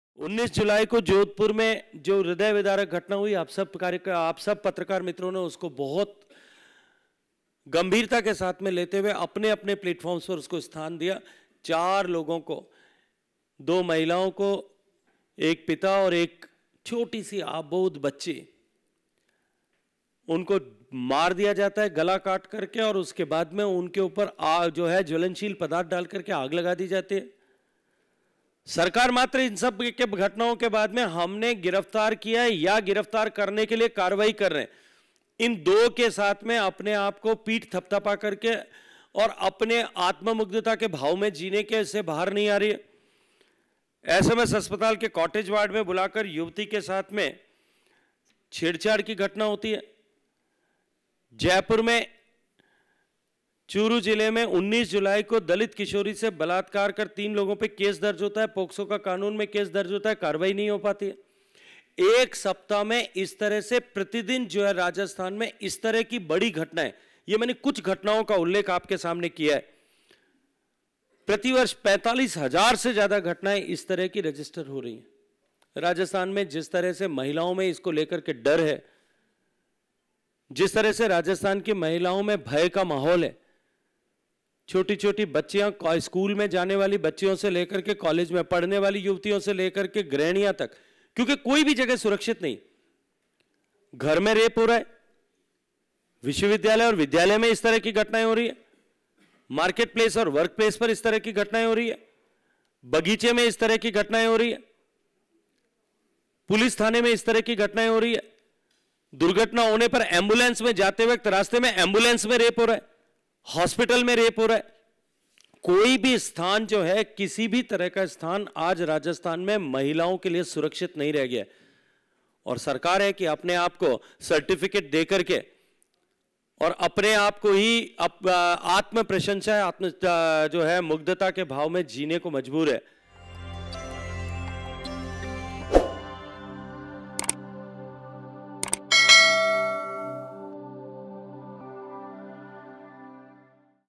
राजस्थान में महिलाओं के लिए कोई भी जगह सुरक्षित नहीं रह गई है | Gajendr Shekhwat | Women Atrocity
राजस्थान में स्कूल जाने वाली बच्चियों से लेकर कॉलेज में पढ़ने वाली युवतियों और गृहणियों तक में डर है। घर, स्कूल, कॉलेज, मार्केट प्लेस, वर्क प्लेस, पुलिस स्टेशन, अस्पताल हर जगह रेप हो रहा है। राजस्थान में महिलाओं के लिए कोई भी जगह सुरक्षित नहीं रह गई है।

- श्री @gssjodhpur
… Show more